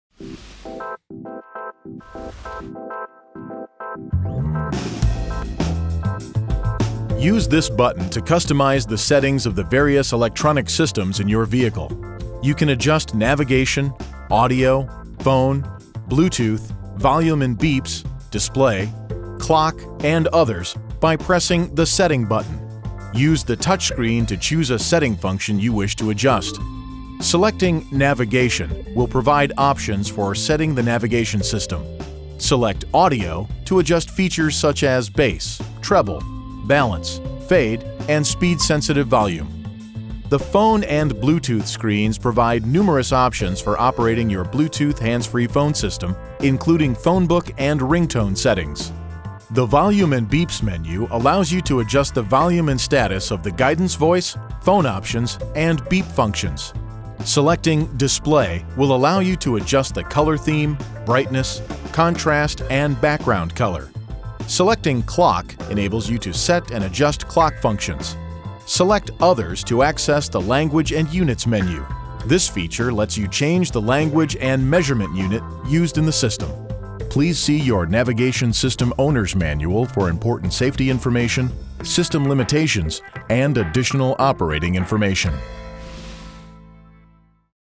2017 Infiniti QX50 - Setting Button (if so equipped)
Use this button to customize the settings of the various electronic systems in your vehicle.

You can adjust Navigation, Audio, Phone, Bluetooth®, Volume and Beeps, Display, Clock and Others by pressing the SETTING button.

Use the touchscreen to choose a setting function you wish to adjust. Selecting NAVIGATION will provide options for setting the Navigation System.
Select AUDIO to adjust features such as Bass, Treble, Balance, Fade and Speed Sensitive Volume.

The PHONE and BLUETOOTH screens provide numerous options for operating your Bluetooth® Hands-free Phone System, including phonebook and ringtone settings.

The VOLUME AND BEEPS menu allows you to adjust the volume and status of the Guidance Voice, Phone options and Beep functions.

Selecting DISPLAY will allow you to adjust the color theme, brightness, contrast and background color.

Selecting CLOCK enables you to set and adjust clock functions.

Select OTHERS to access the LANGUAGE AND UNITS menu. This feature lets you change the language and measurement unit used in the system.

Please see your Navigation System Owner’s Manual for important safety information, system limitations and additional operating information.